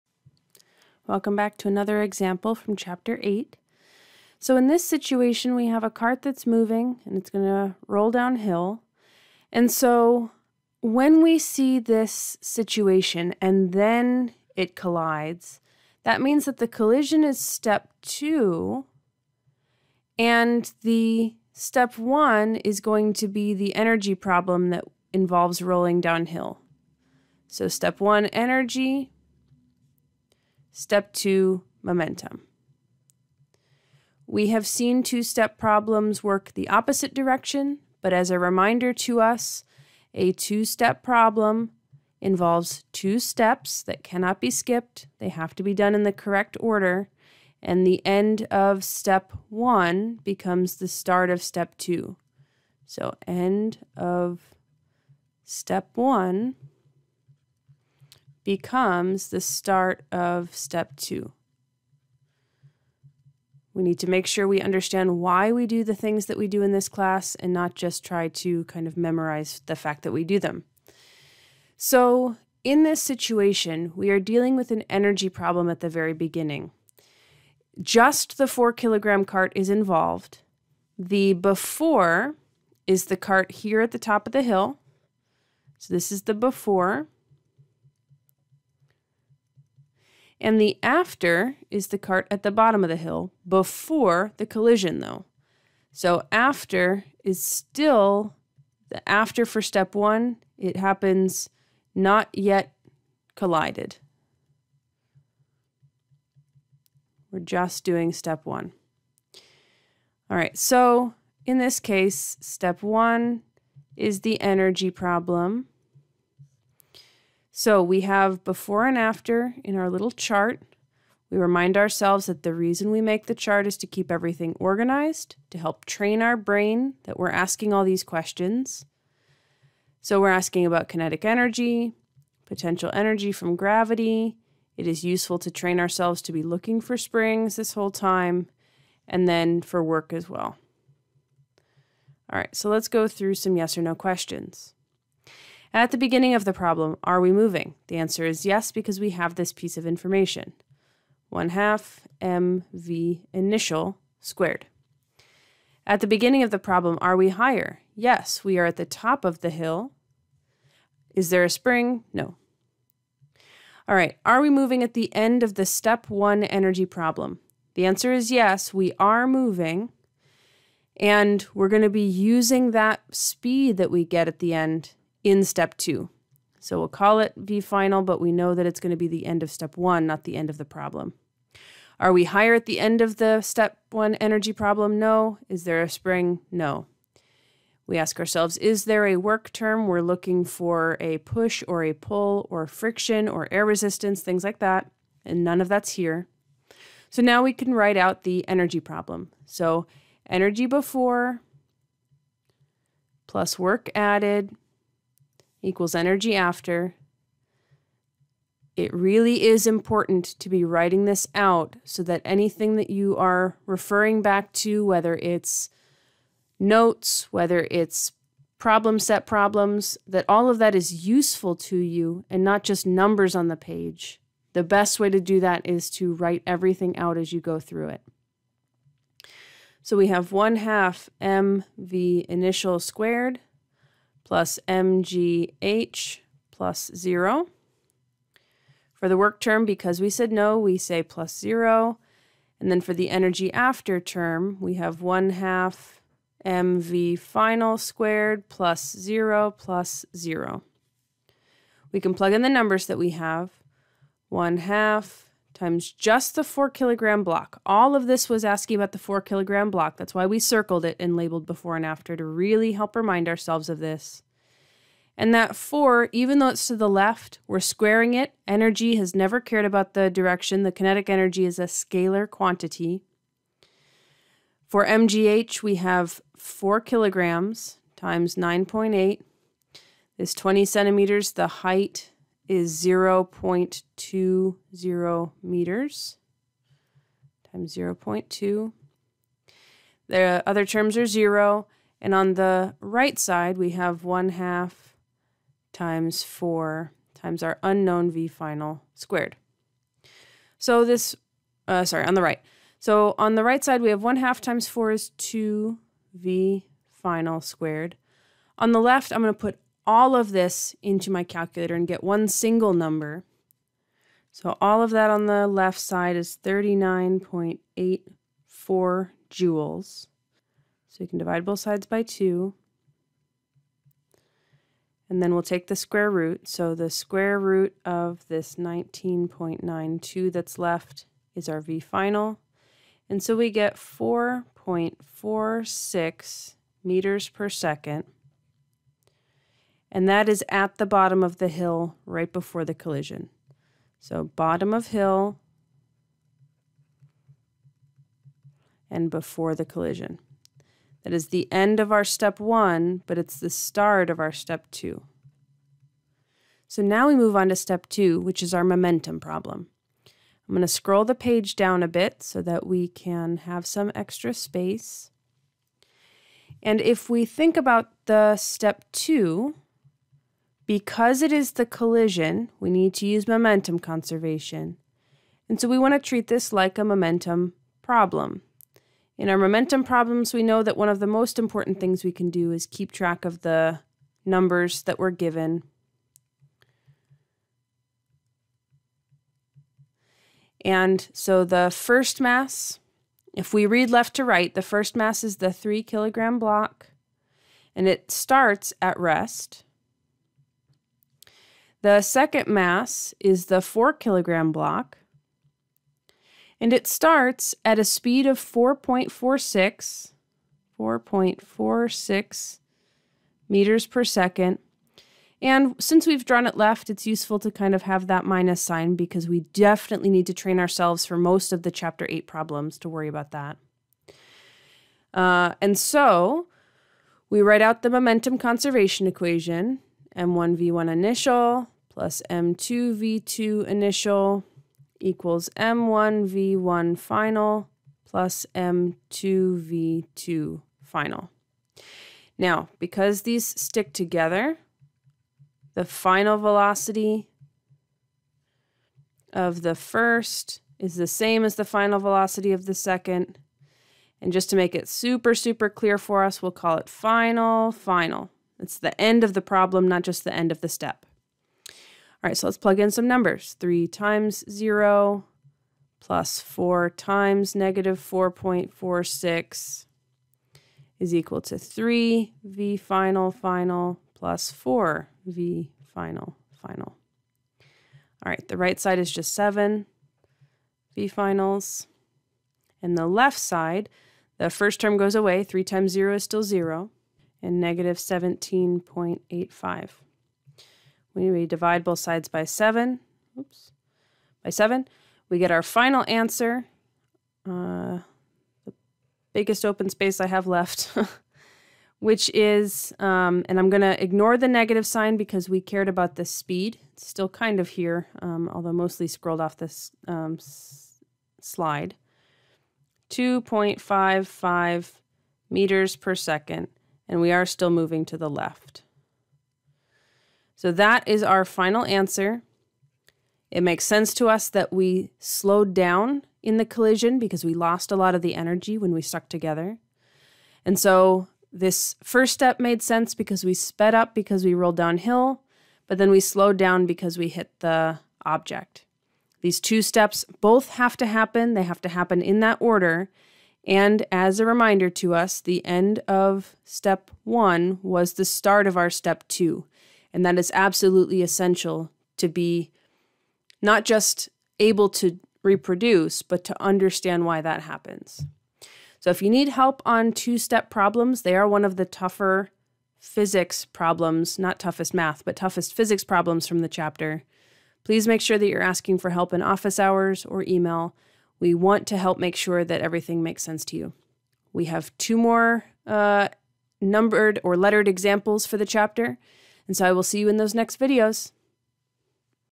GRCC Physics 125 - Online Lecture - Example 8F
This is an example from Openstax Chapter 8 (Momentum and Collisions). It is intended to be watched after the introductory lecture videos for this chapter.